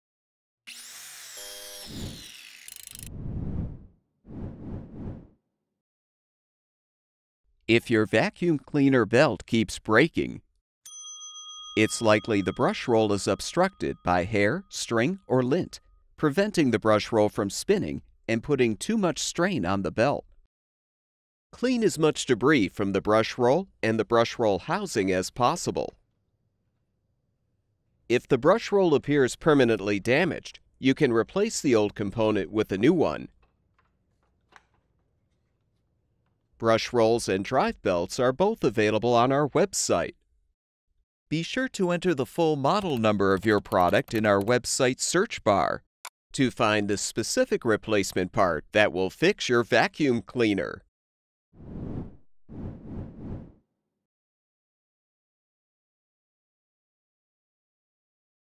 Vacuum Cleaner Belt Keeps Breaking — Vacuum Cleaner Troubleshooting
Vacuum cleaner belt keeps breaking? This video provides information on how to troubleshoot a vacuum cleaner and the most likely defective parts associated with this problem.

Commonly replaced vacuum cleaner parts:

Air Filter
Belt
Touchpad
Fan Blade
Blower Wheel
Brushroll
Cannister
Wheel
Housing
Fan Motor